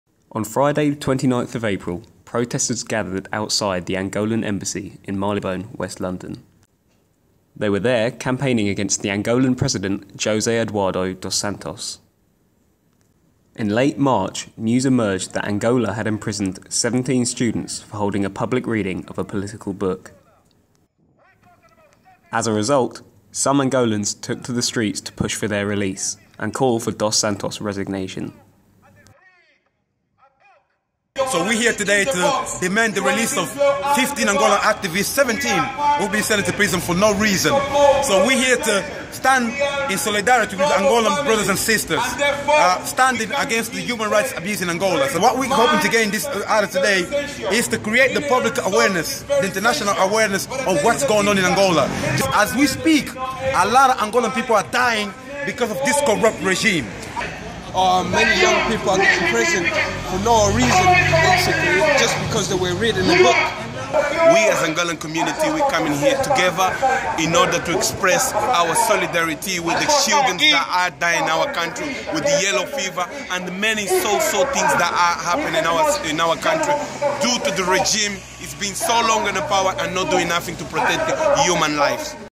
Protesters march on Angolan embassy
Demonstrators call for the release of 17 Angolan students at the Angolan embassy in London.